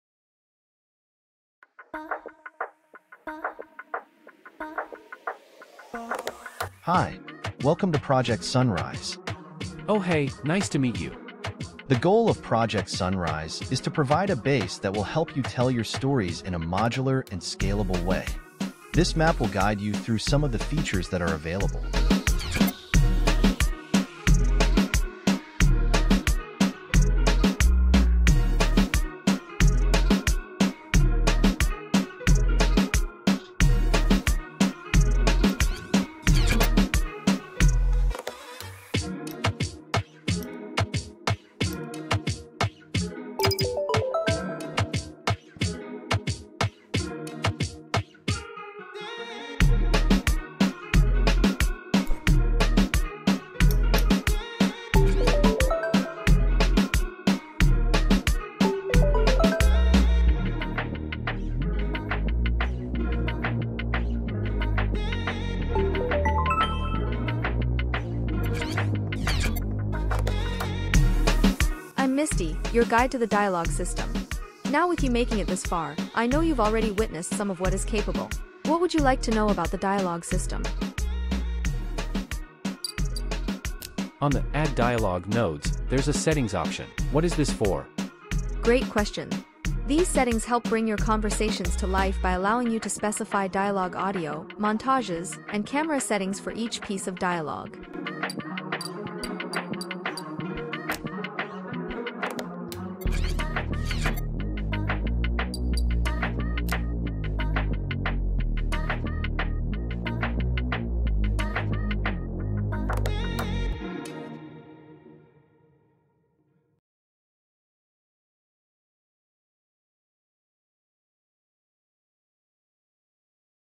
UE5 - Project Sunrise - Dialogue, Interaction & Quest System - Showcase
This is the official showcase of Project Sunrise, a blueprint-only project designed to accelerate and simplify your development workflow. This video highlights key features and functionalities aimed at easing initial setup and providing transparency for easy modifications.

Project Sunrise is available on the FAB Marketplace and is a project to not only save you time but give you a solid base to build your future projects from.

Be sure to check out the playable demo at the URL below.

If you want to support me, why not conciser making a donation?